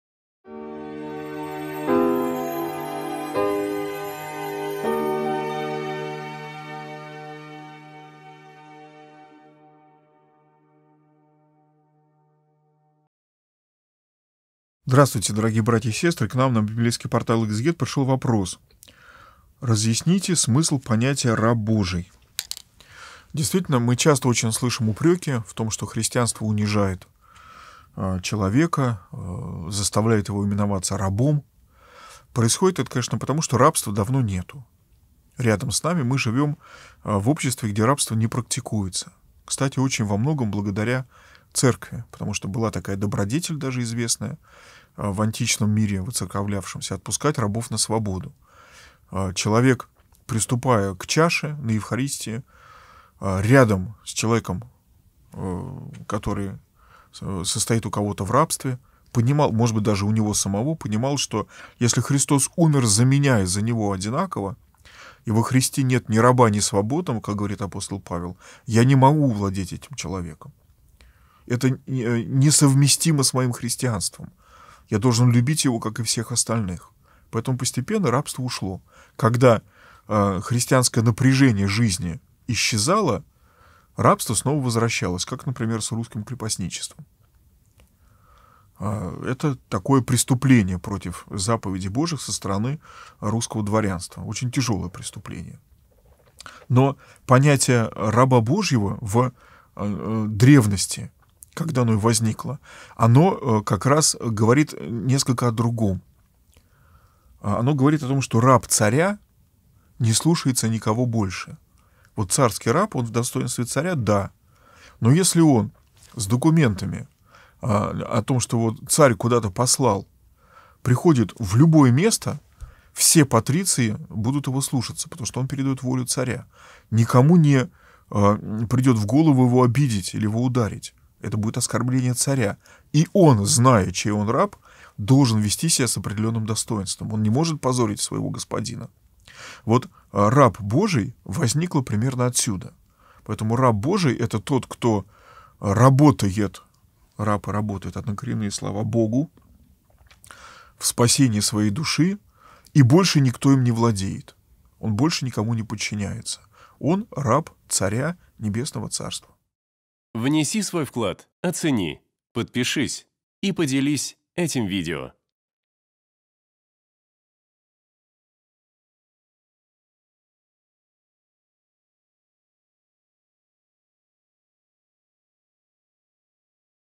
РАБ БОЖИЙ. Протоиерей Феодор Бородин
Команда Экзегет.

На библейском портале Экзегет вместе со священником Федором Бородиным рассуждаем на тему: "Раб Божий."

🔸  Античная традиция отпускать рабов на свободу
🔸  Крепостное право – преступление заповедей Божиих
🔸  Раб Божий  –  тот, кто работает Богу